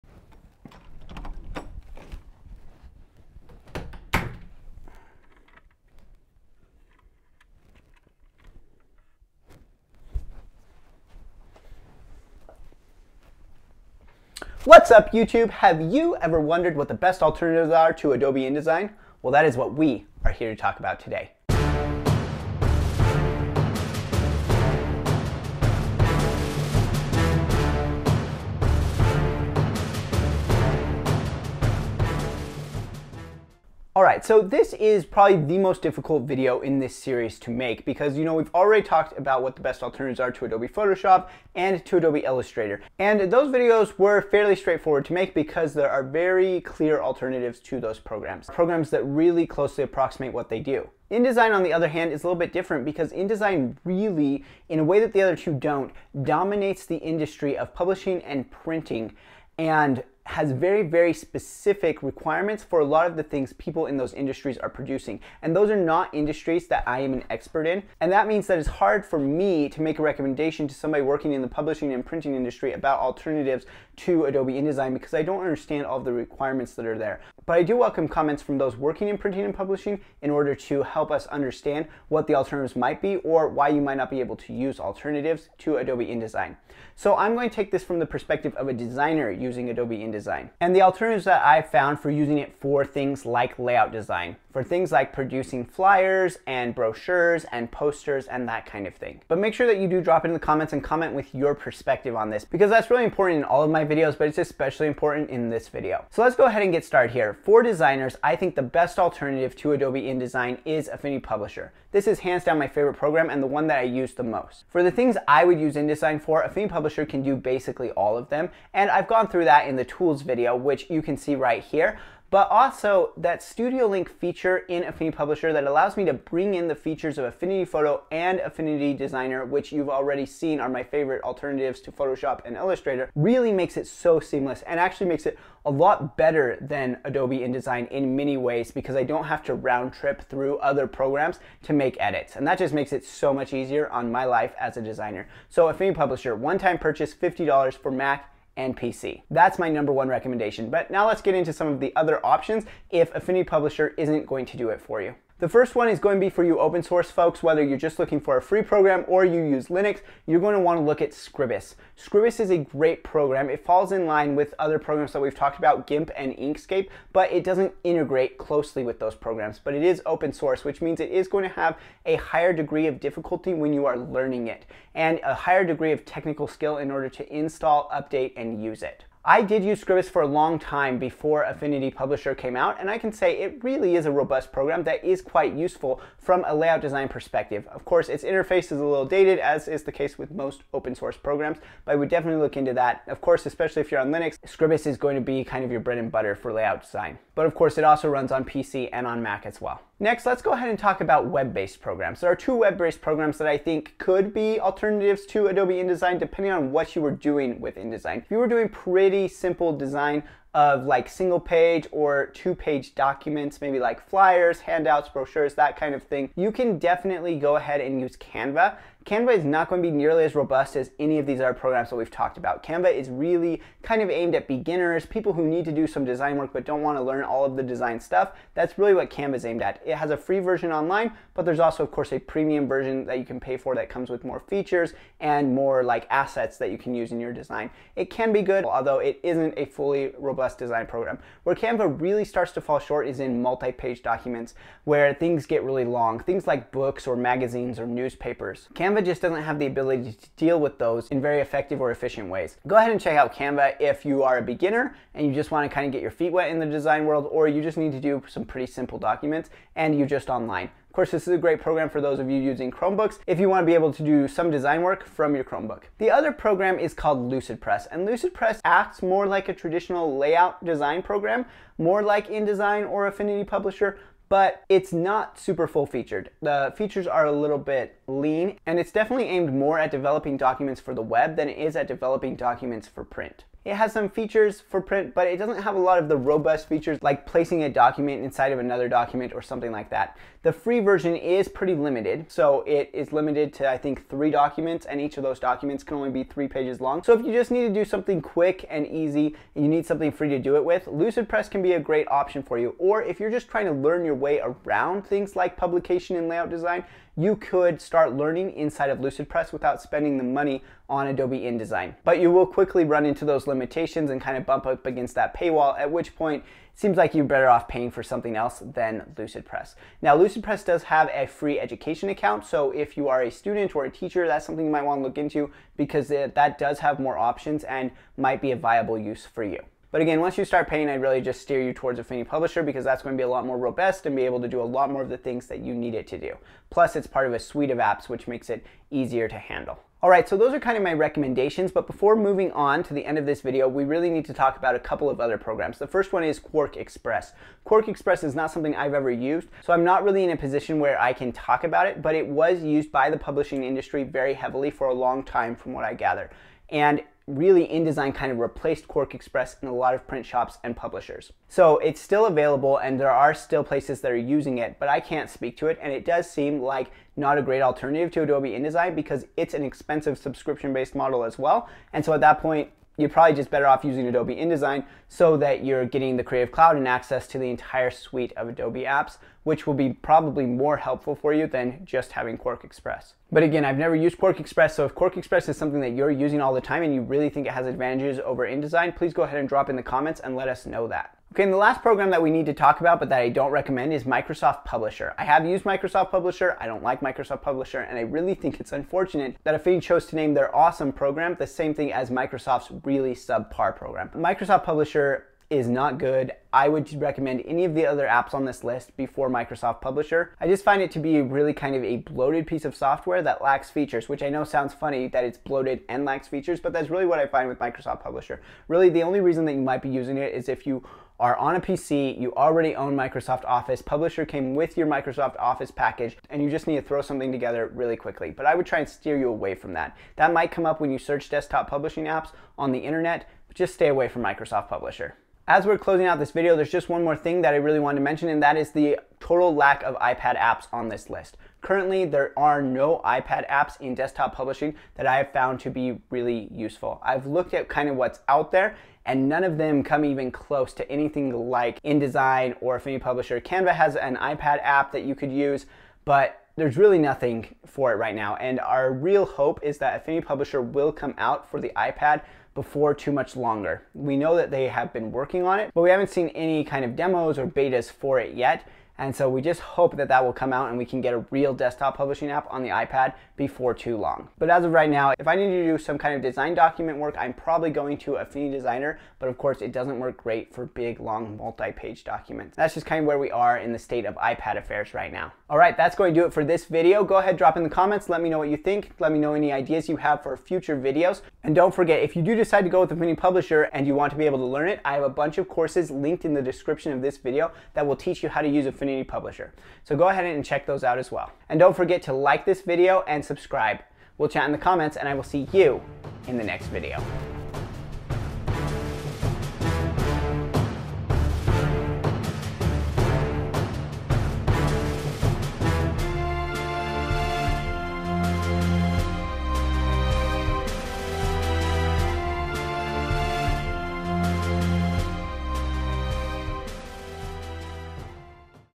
Best InDesign Alternatives
In this video we talk about what I think the best alternatives to Adobe InDesign are.

Music is by Ben Sound